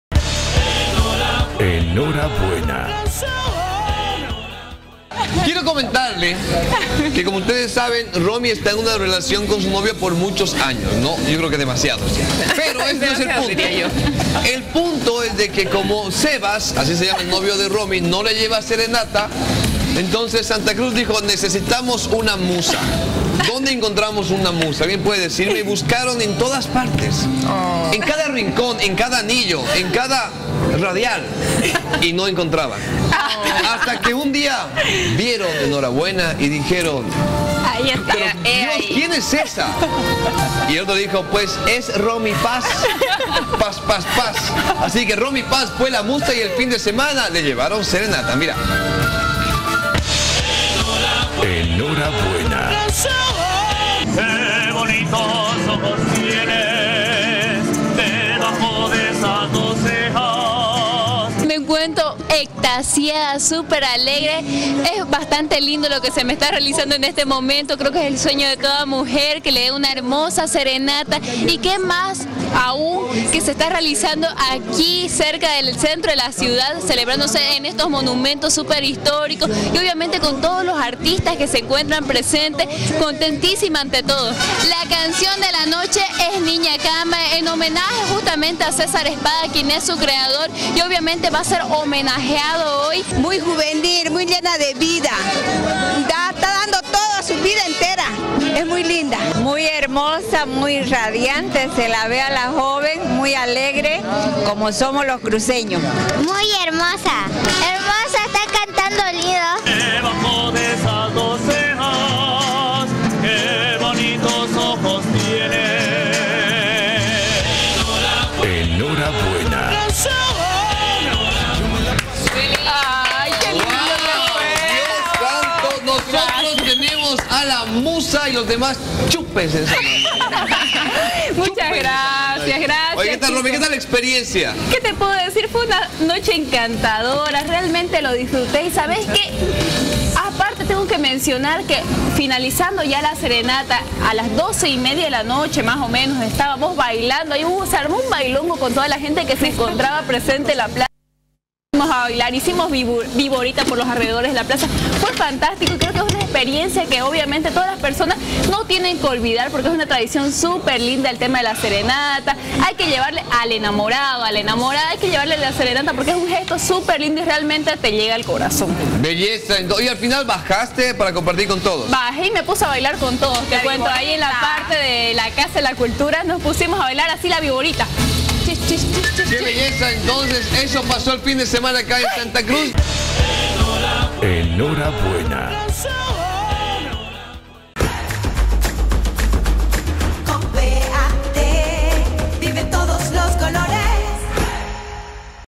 Romy Paz musa del “día mundial de la serenata”
Romy Paz presentadora de Enhorabuena fue musa del “día mundial de la serenata”.